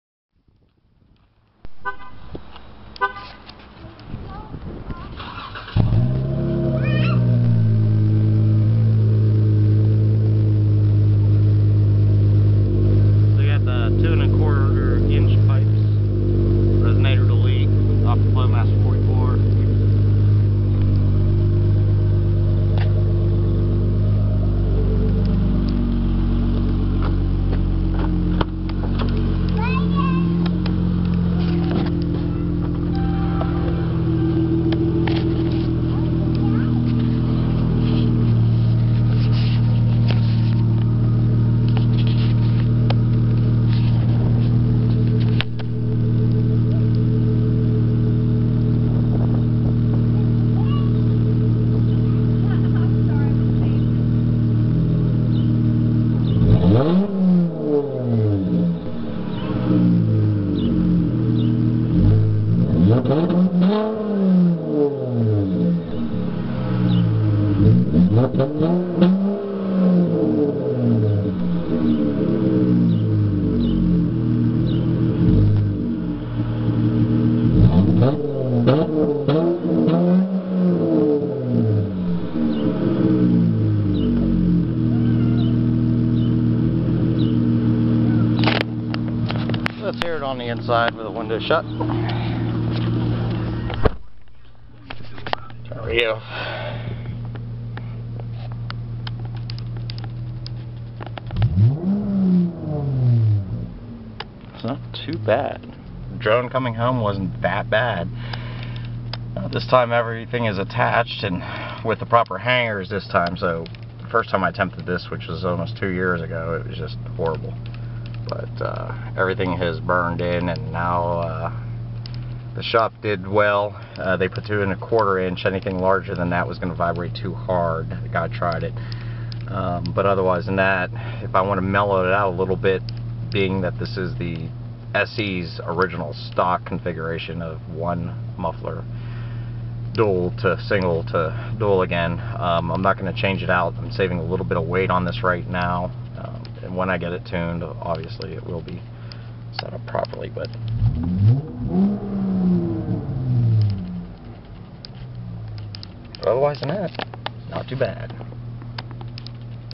Dodge Charger V6 3.6L Pentastar Flowmaster
Dodge Charger V6 3.6L Pentastar Flowmaster Super 44 and reso deletion. I finally reverted back to my original. Those resonators are heavy for what they are. I went with 2.25" pipes to say continuous on the rear setup, but also the 2.5" got a little rattle that was annoying.